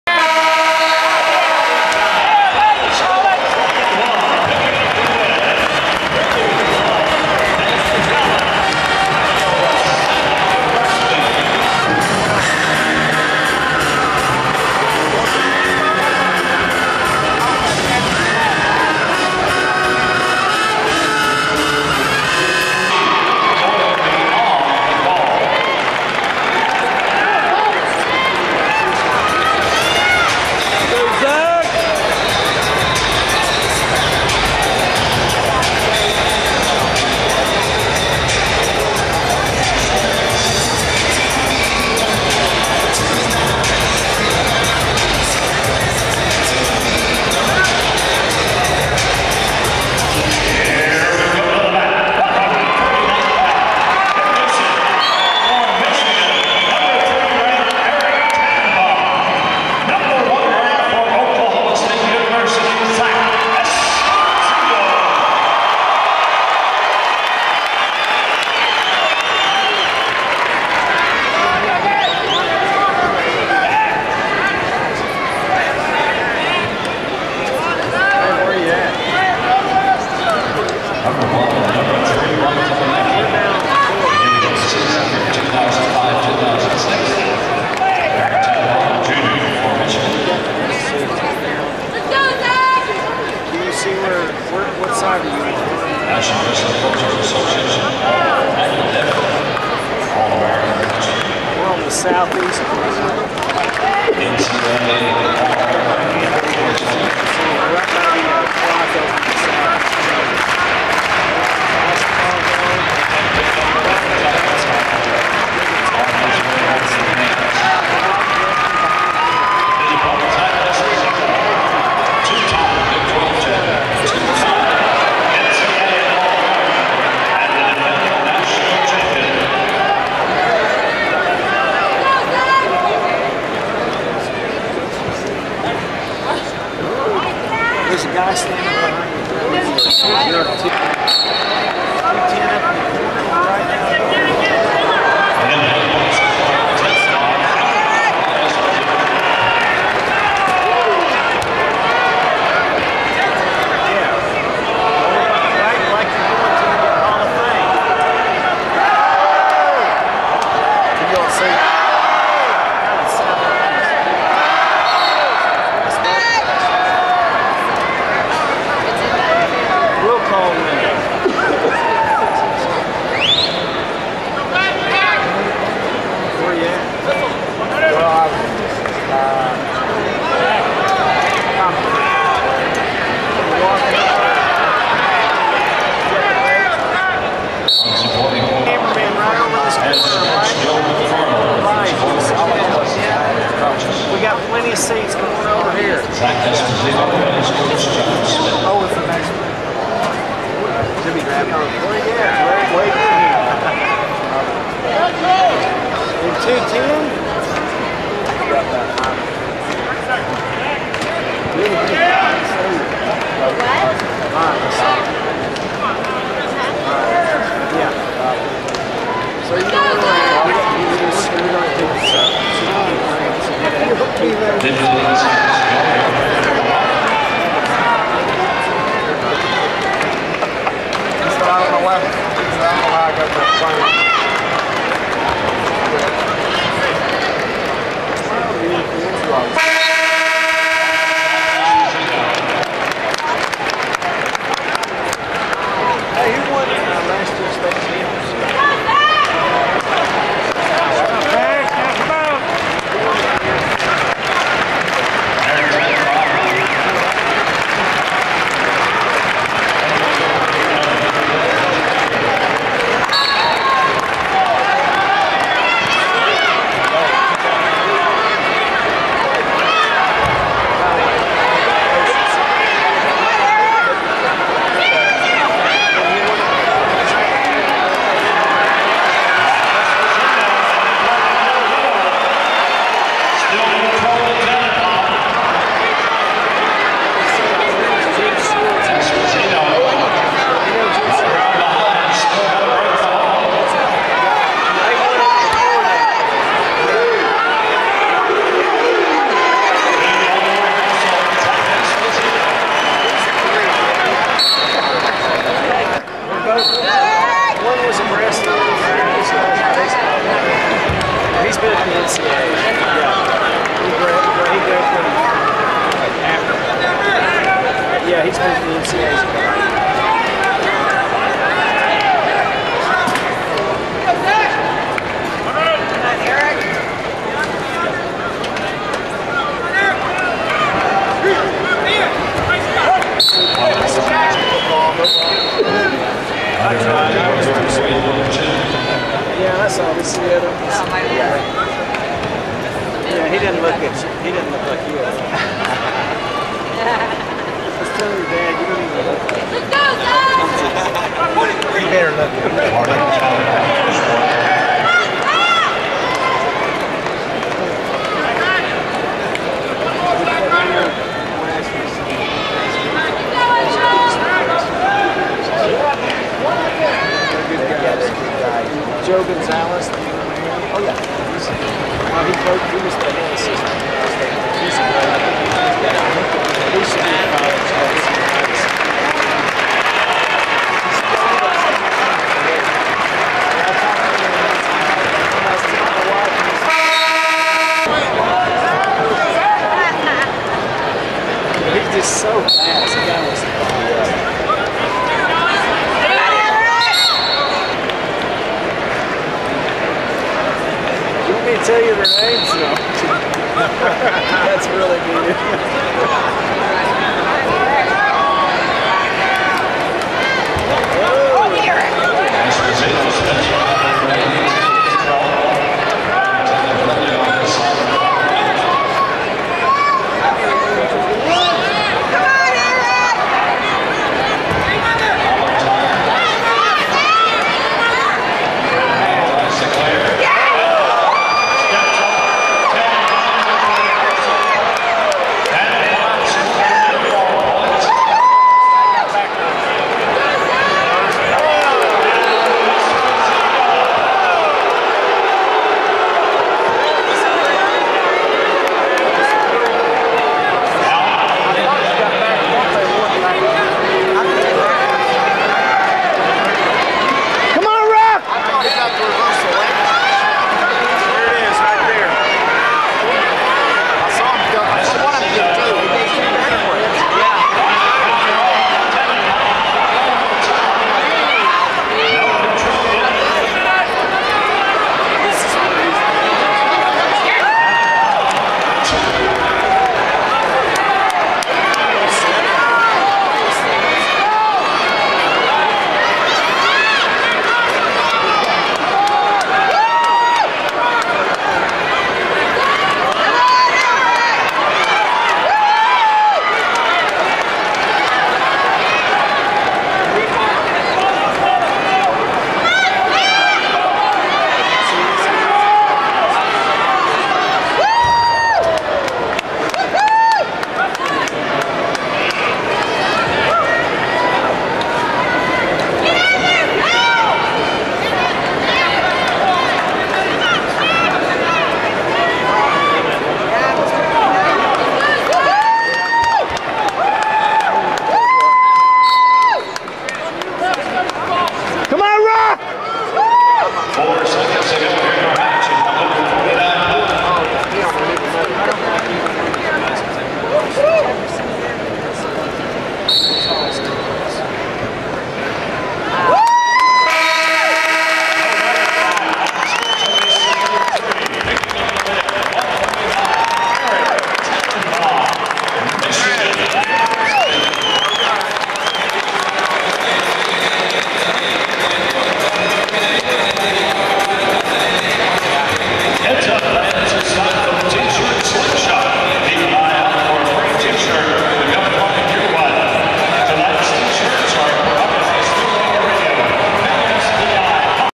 Eric Tannenbaum (Michigan) vs Zack Esposito (OSU) Wrestling All Star Match
Eric Tannenbaum (U. Mich) vs. Zack Esposito (Oklahoma State) in the Wrestling All Star Classic